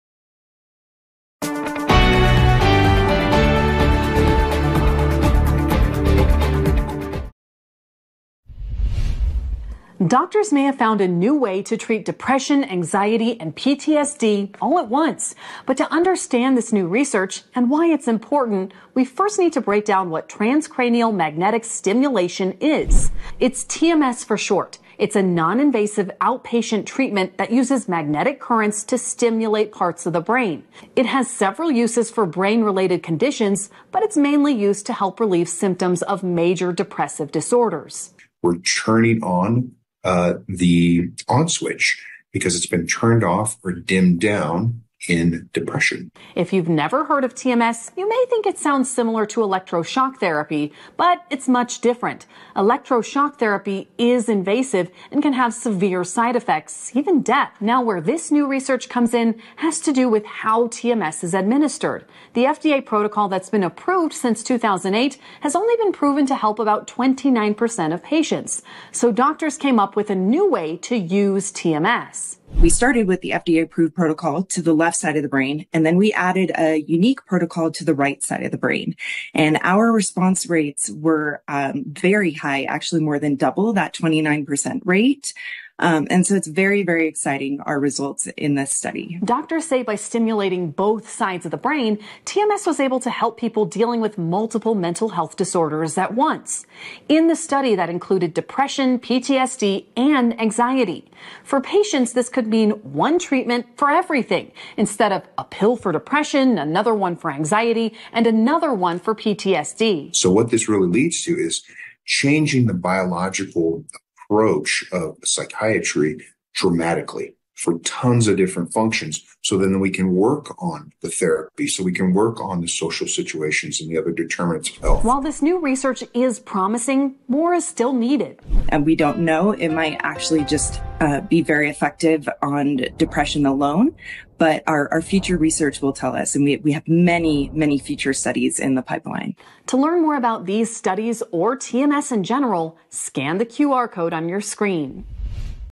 New TMS Method Offers Hope for Patients with Hard-to-Treat Depression
TMS, or transcranial magnetic stimulation, is an advanced, non-invasive treatment for depression, OCD, anxiety and more. It's a proven, highly effective option for patients who haven't responded adequately to antidepressants or other treatments.

In a recent Family Care Center study, researchers found that by making slight modifications to an already approved TMS technique, patients with hard-to-treat depression experienced profound improvement. Overall, 66% of patients with major depressive disorder who took part in the study responded to the novel treatment.

Family Care Center's chief medical officer, Dr. Chuck Weber, and director of research, Dr. Sabrina Segal, discussed the study with KOAA TV in Colorado Springs.